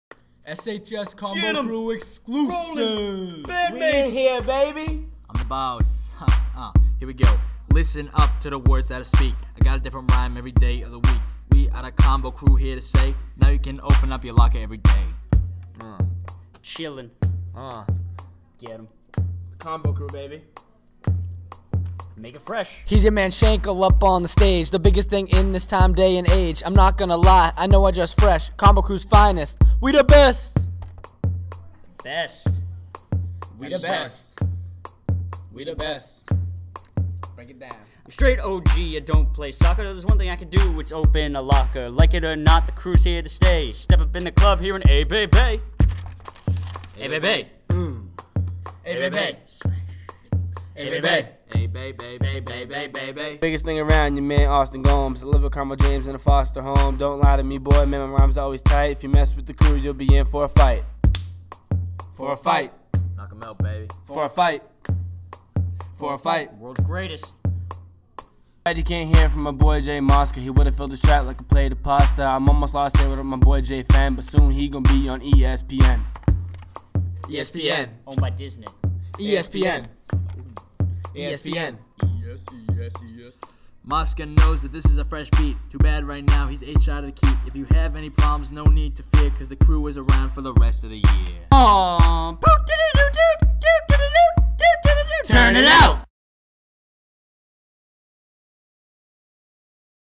Combo Crew - Lockers Episode 5
5 of 5 the rap

Ross: (I'm the boss, uh, here we go.)
Listen up to the words that i speak
I got a different rhyme every day of the week
We are the Combo Crew here to say,
now you can open up your locker every day.

Brent:
heres your man shenkel up on the stage,
the biggest thing in this time, day, and age.
Im not gonna lie, i know i dress fresh,
combo crew's finest - we the best

harry:
Im a straight og, i dont play soccer,
if theres one thing i can do, its open a locker.
like it or not the crews here to stay,
step up on the club yellin ay bay bay

gomes:
biggest thing around, yo man austin gomes
i live with carmel james in a foster home
dont mess with me boy, you know my rhymes are so tight.
if you mess wit the crew youll be in for a fight.

gomes:
too bad you cant hear from my boy j.mosca,
he woulda filled this track like a plate full of pasta.

im almost as good as my boy j.fen,
soon he gon be on ESPN.

Ross:
mosca knows that this is a fresh beat,
too bad right now he's h'd out the keat.
if you have any problems, no need to fear
cause the crew is around for the rest of the year